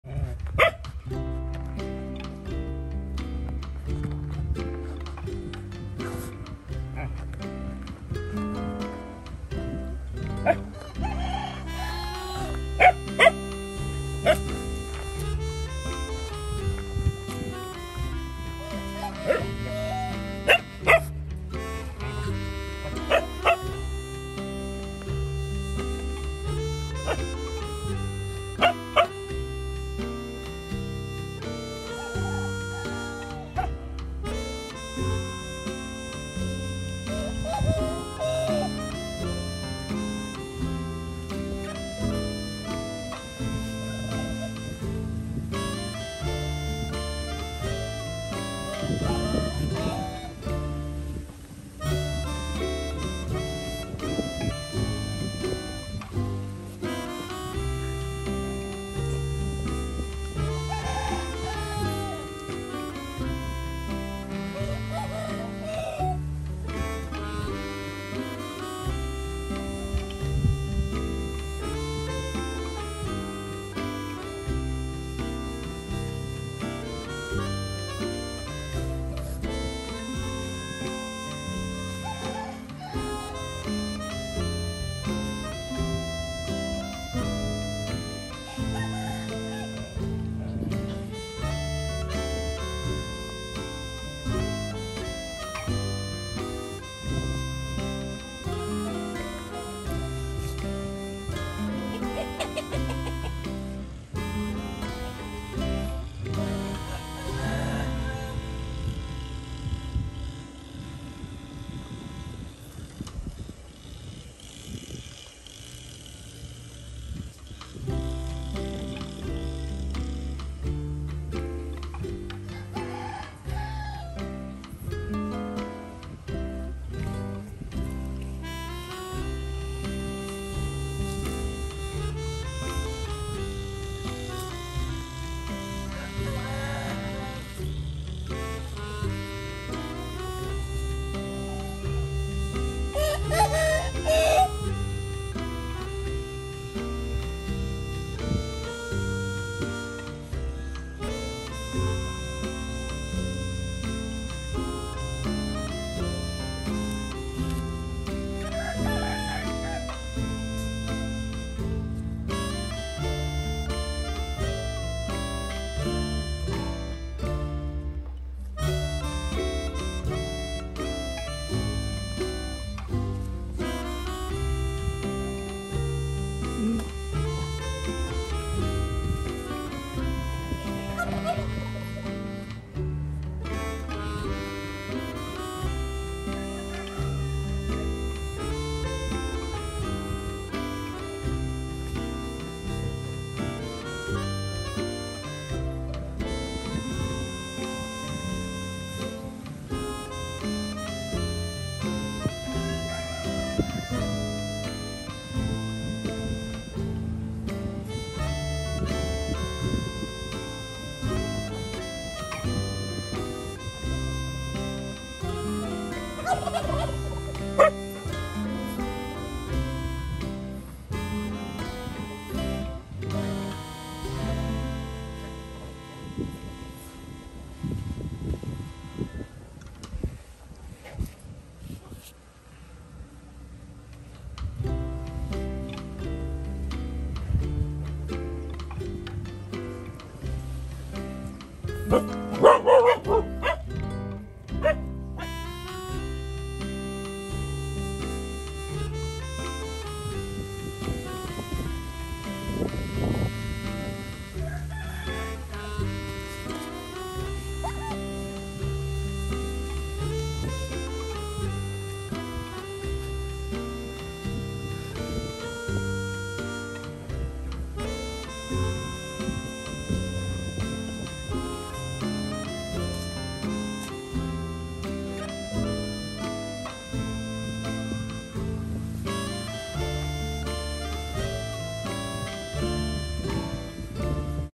Reaction of otter when otter play with dad | Moon Na
Moon Na
Otter has good time with dad, look so happy and adorable 🥰.